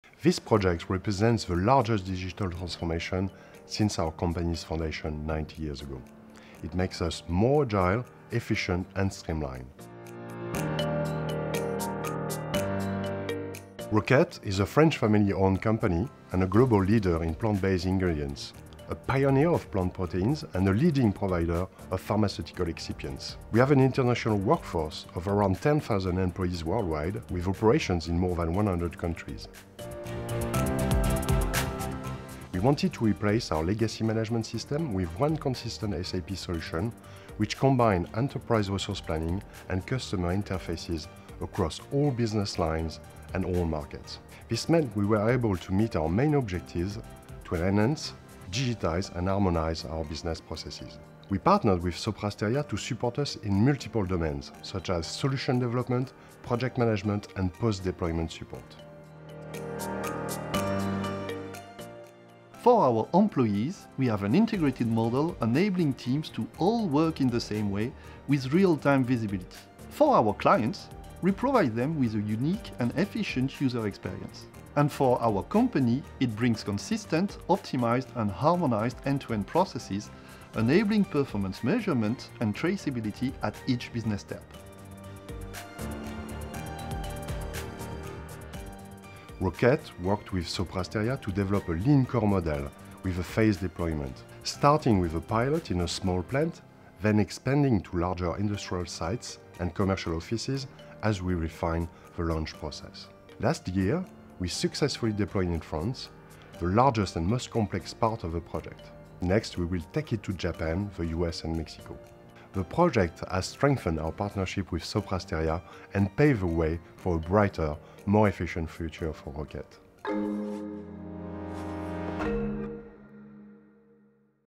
New data infrastructure, new future
Roquette Chief Digital and Information Officer Michael Manouvrier and Head of Digital Business Solutions Benjamin Paris explain how the company replaced its outdated legacy data infrastructure with a new cutting-edge SAP solution to break down silos, harmonise business processes and boost visibility company-wide.